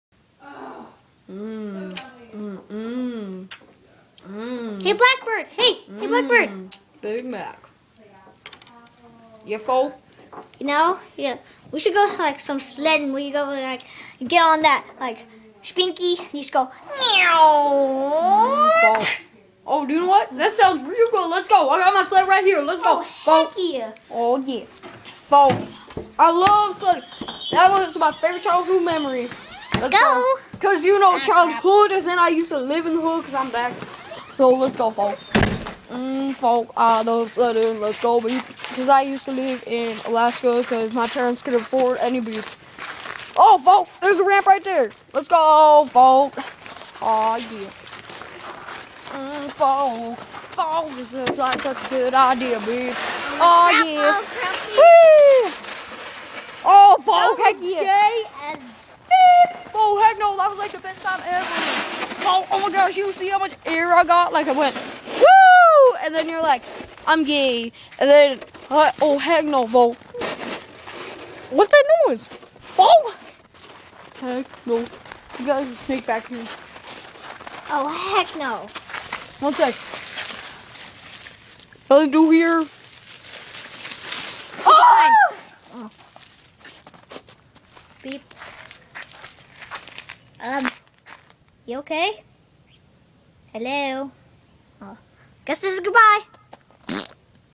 Angry Birds Sledding
Philipe and black bird go sledding!